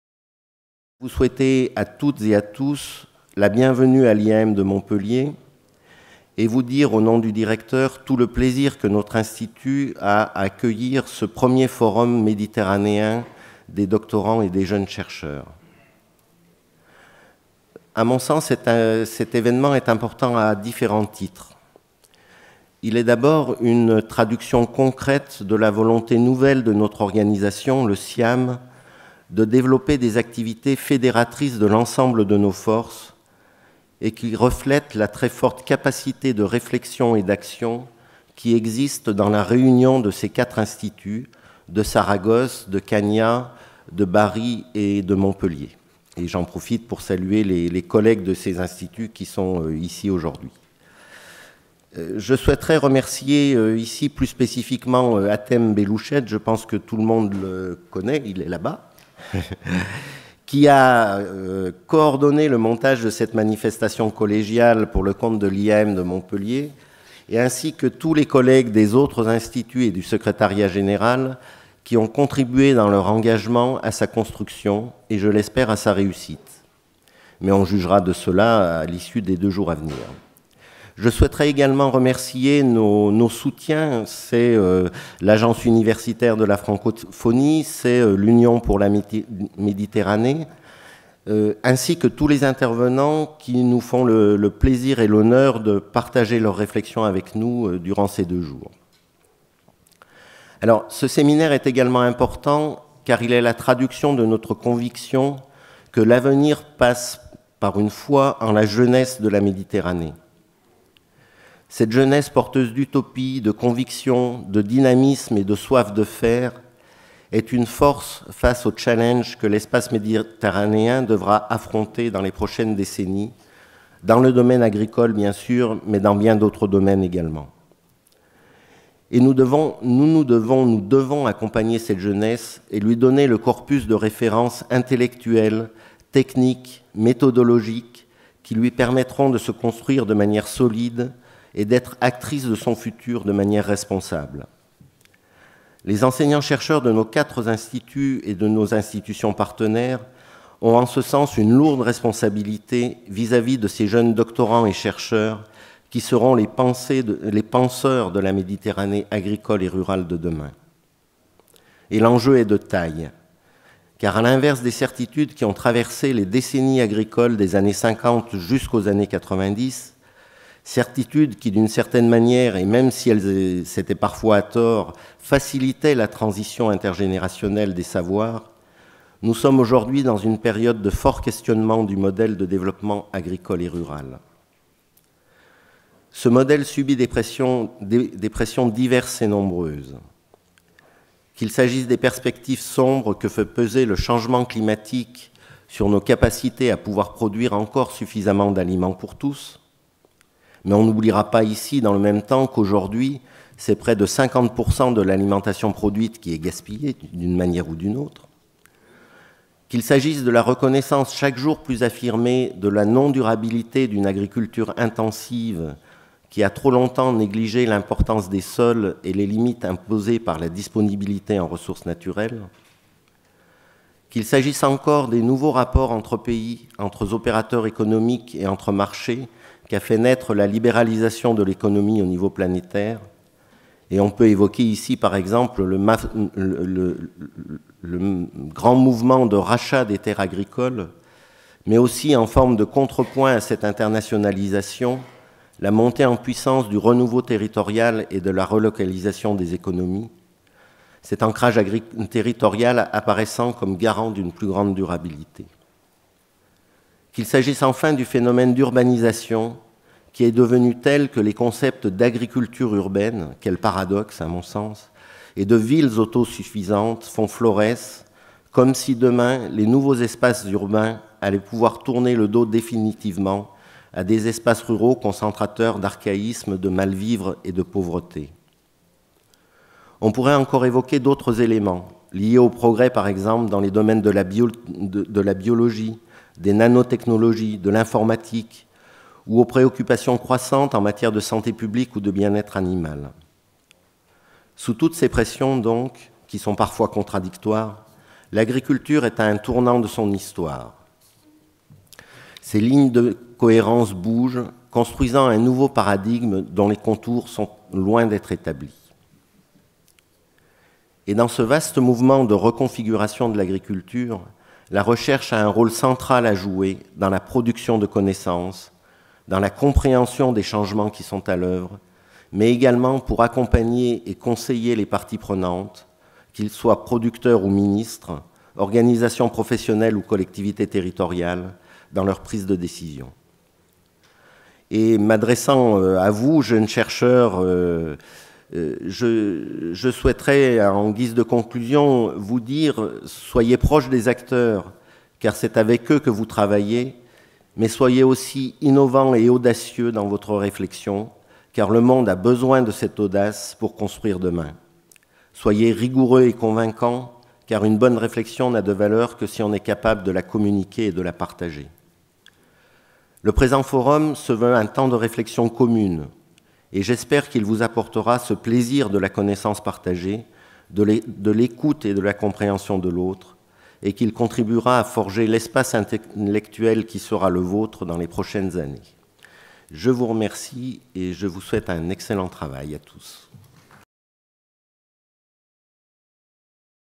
MedForum CIHEAM 2016: Introduction Jean-Paul Pellissier
Le CIHEAM a organisé le 1er Forum sur la «conception de systèmes de production agricole et alimentaire durables» à l'intention des doctorants et jeunes chercheurs de la région méditerranéenne, afin de favoriser la communication, le dialogue et la publication entre les différents partenaires de la recherche scientifique dans le domaine du développement rural de ces pays.

85 participants selectionnés issus de 10 pays méditerranéens,
40 présentations orales et plus de 60 posters ont accompagnés les débats d'experts consacrés aux opportunités et difficultés des jeunes chercheurs des Pays Méditerranéens.